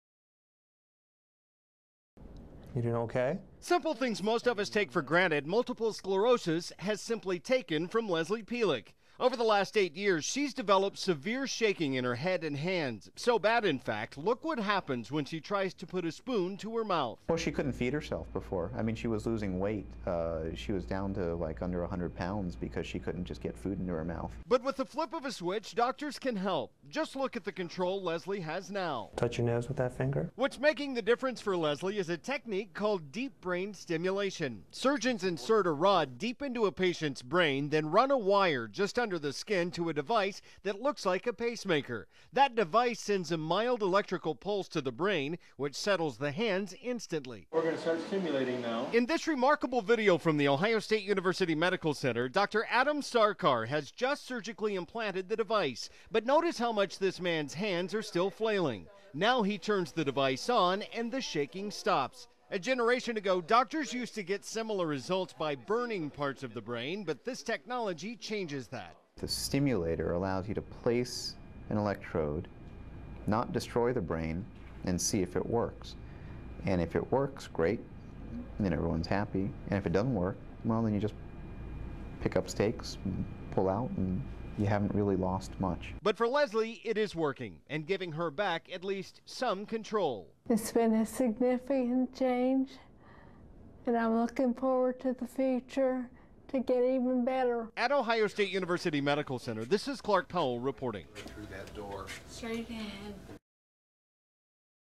Implant In The Brain Calms Shaky Hands
On average, another person is diagnosed with multiple sclerosis every hour in the U.S.  MS is a devastating disease that can slowly rob patients of everything from their sight to control of their muscles.  Doctors aren't sure what causes it, but they're finding better ways to tame it.  They're placing electronic probes in the brain -- and giving some desperate patients relief.